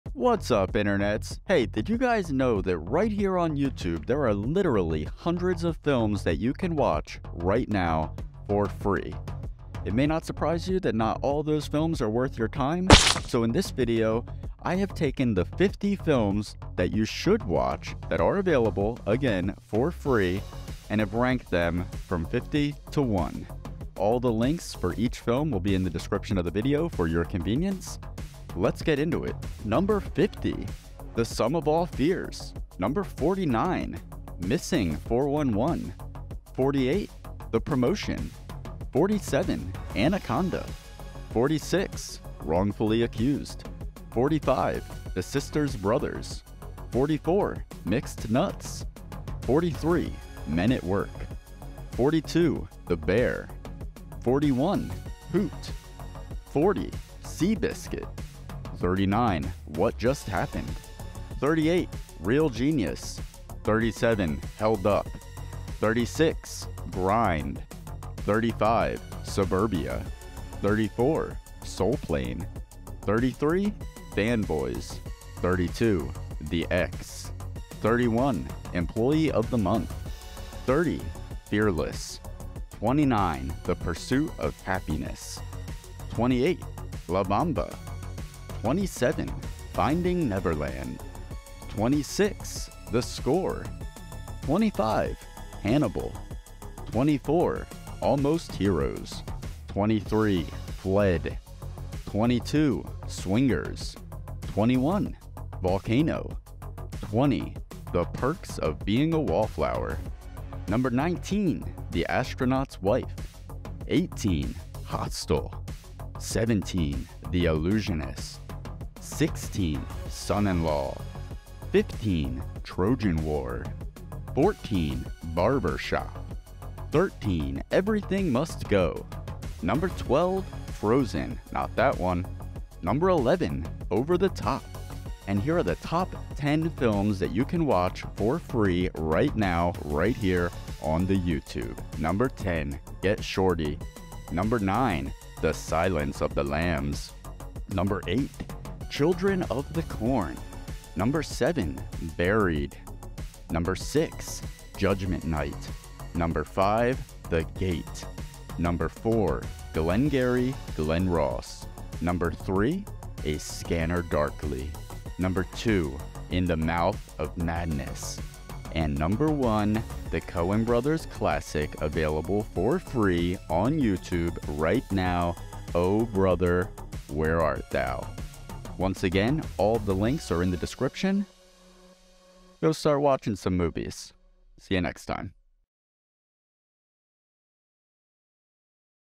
50 Films You Can Watch FOR FREE Right Now on YouTube
YouTube boasts a pretty impressive library of films that you can watch for free and we've went through every single one of them to give you the top 50, ranked.

Each film is linked below in alphabetical order along with a few bonus ones: